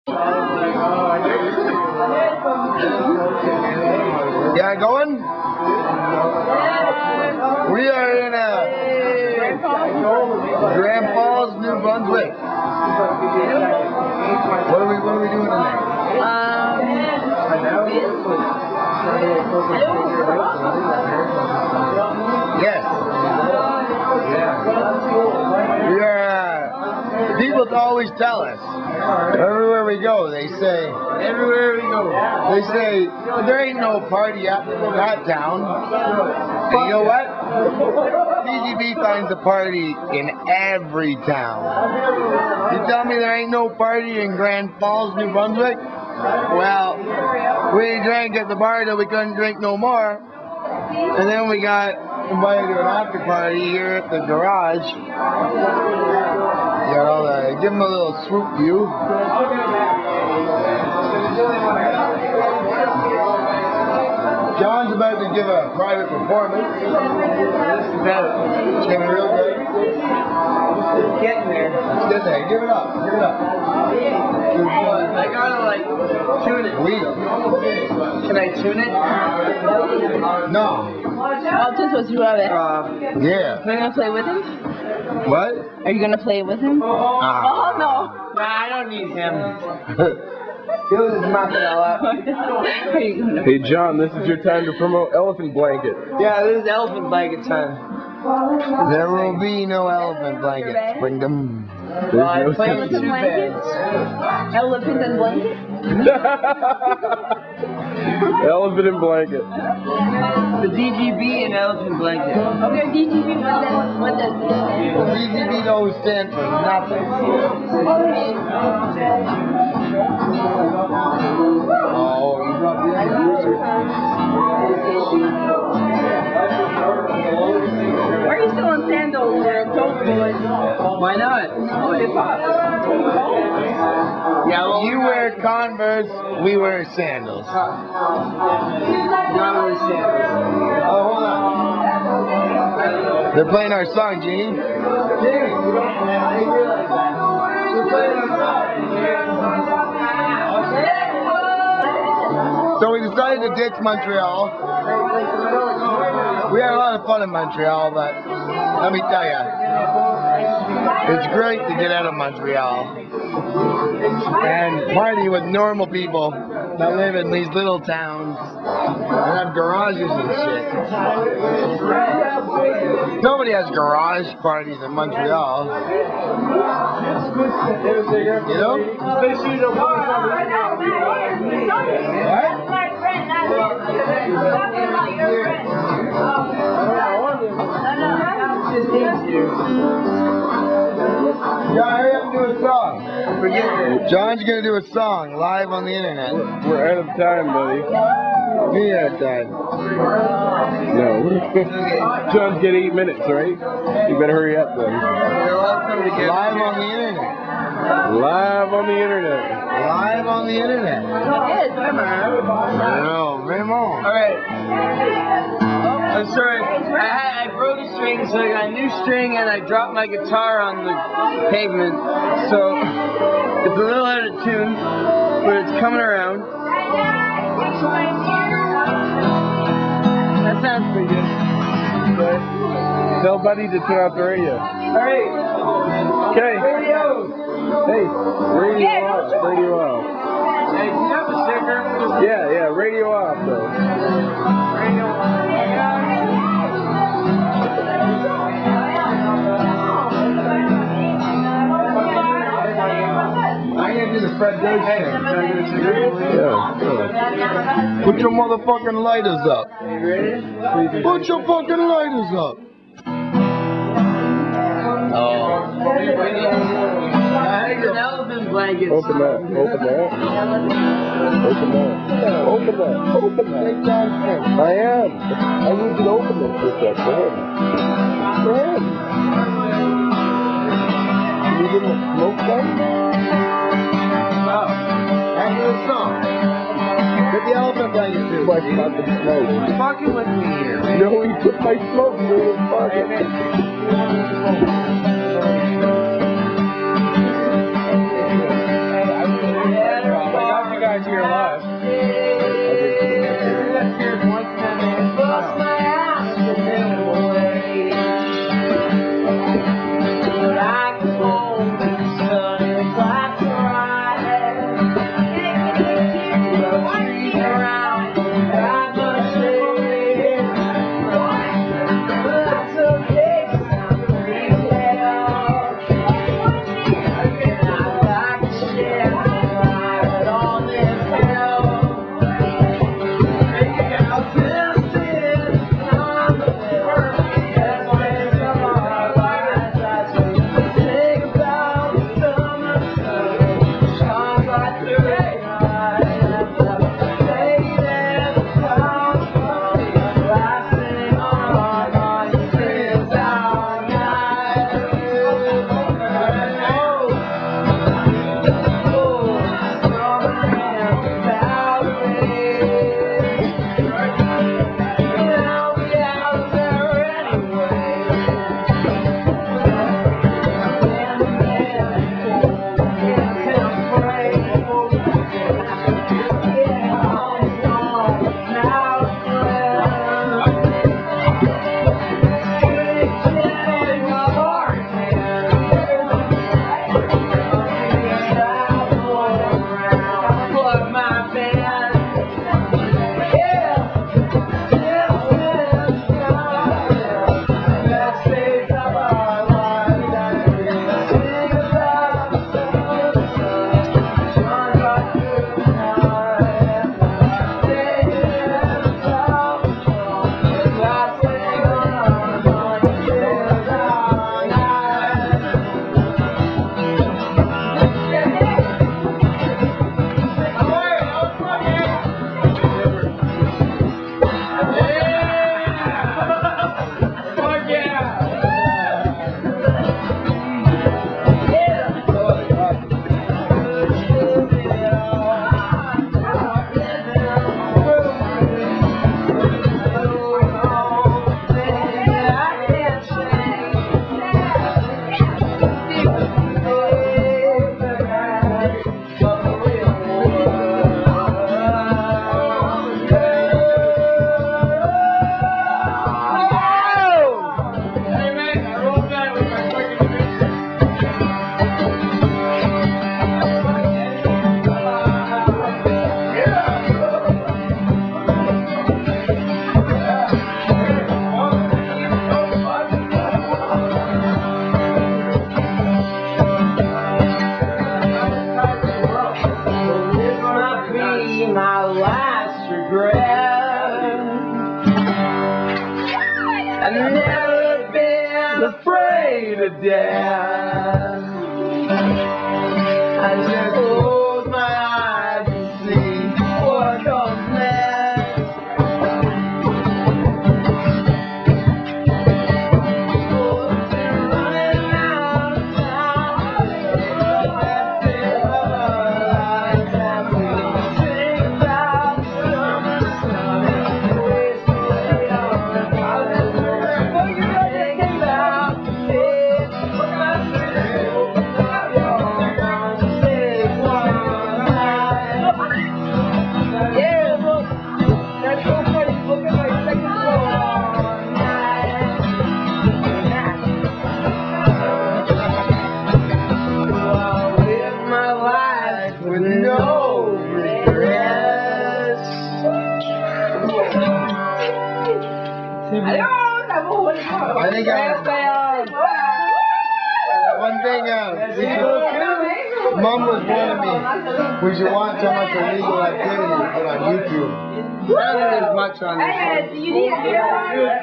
Grand Falls Video Blog - the DGB always finds the party!
Garage party in Grand Falls, New Brunswick! Jon Greer solo performance!